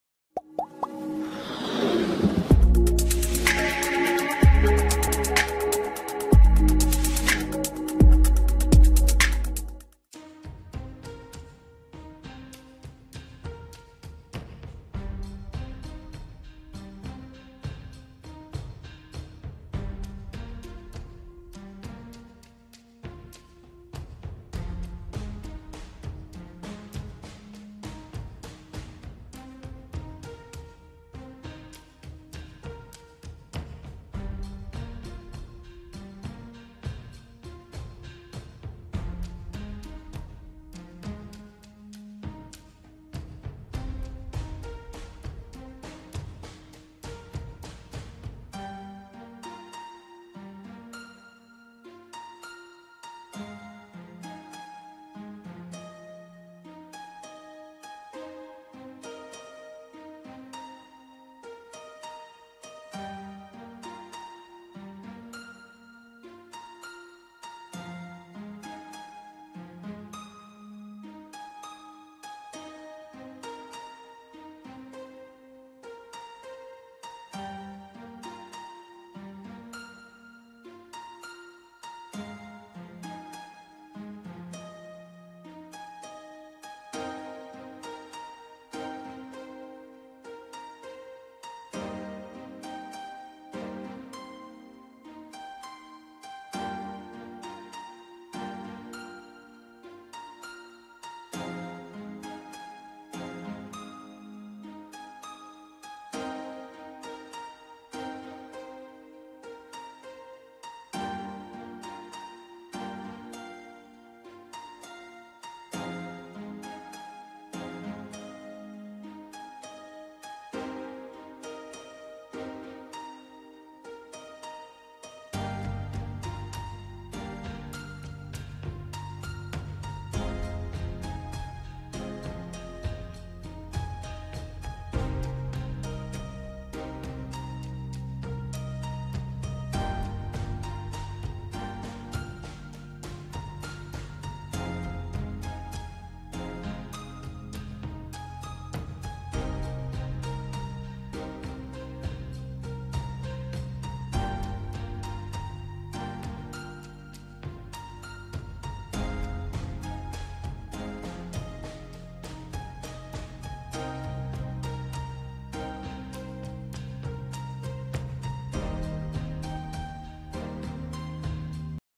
Love is in the air anticipazioni dal 15 al 20 novembre: Eda vuole lasciare Istanbul, Serkan ha un tu
Love is in the air anticipazioni dal 15 al 20 novembre: Eda vuole lasciare Istanbul, Serkan ha un tumore Grandi colpi di scena nelle puntate di Love is in